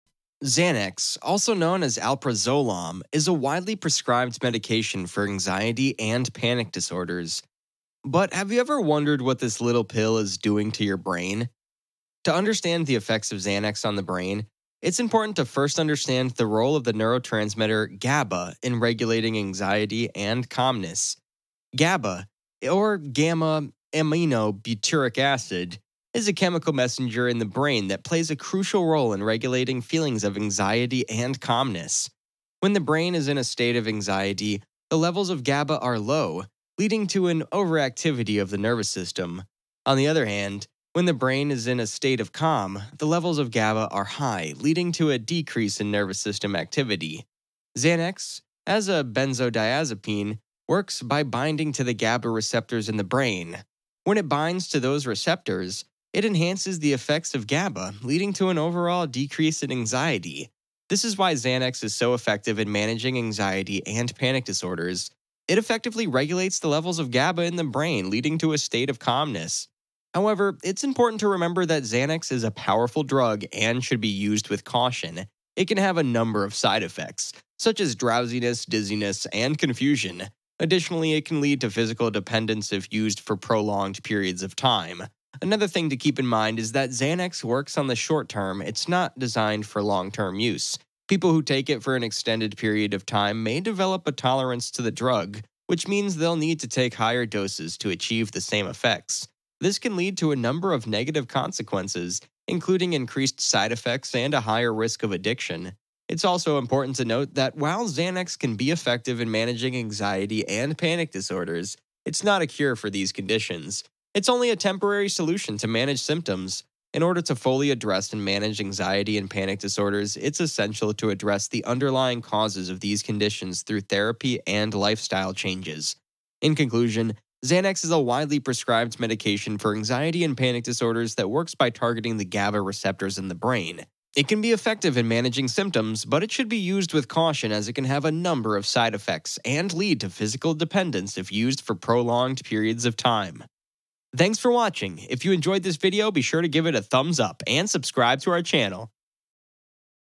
Your Brain On Xanax
In this video, we explore the effects of Xanax on the brain. Xanax is a commonly prescribed medication for anxiety, but its effects on the brain can be complex and potentially dangerous. We dive into the science behind how Xanax alters brain chemistry and the ways in which it can affect mood, cognition, and behavior. We also discuss the potential benefits and risks of using Xanax and offer tips for safe and responsible use. Whether you're considering taking Xanax for anxiety or are simply curious about how this medication affects the brain, this video offers valuable insights and information.